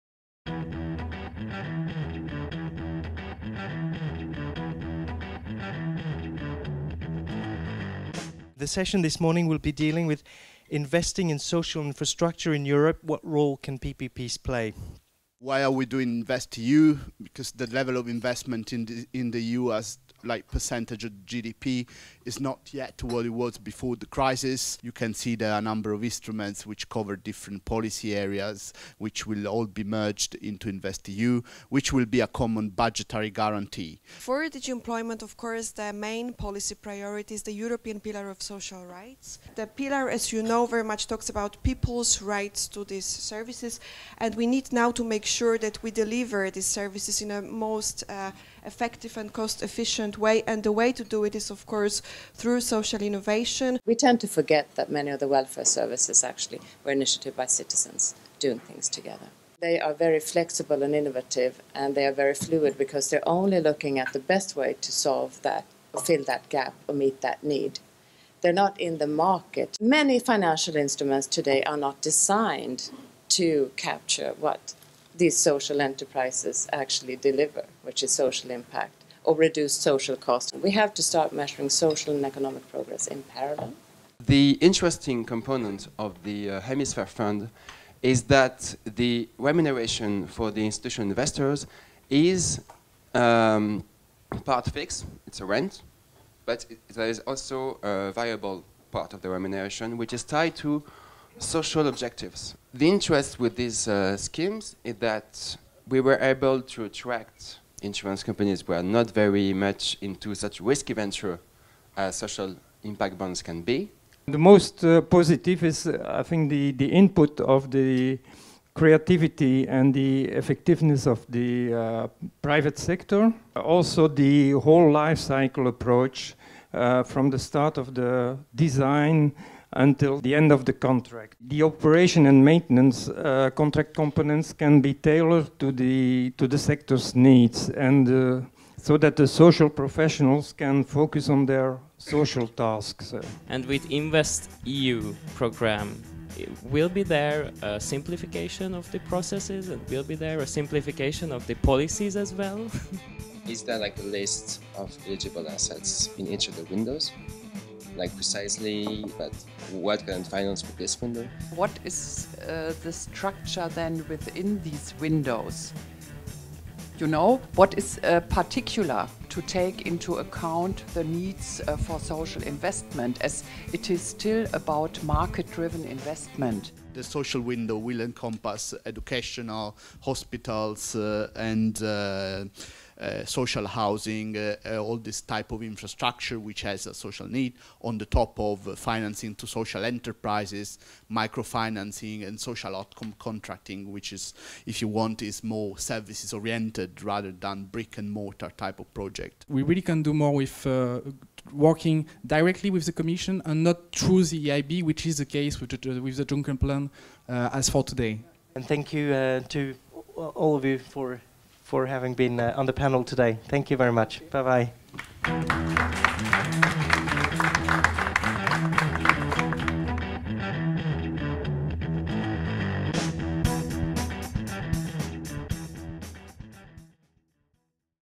Policies & Practices debate on the role of PPPs in social investment
Investing in social infrastructure in Europe: what role can PPPs play? that was the question addressed to a panel of 5 speakers from the European Institutions and organisations.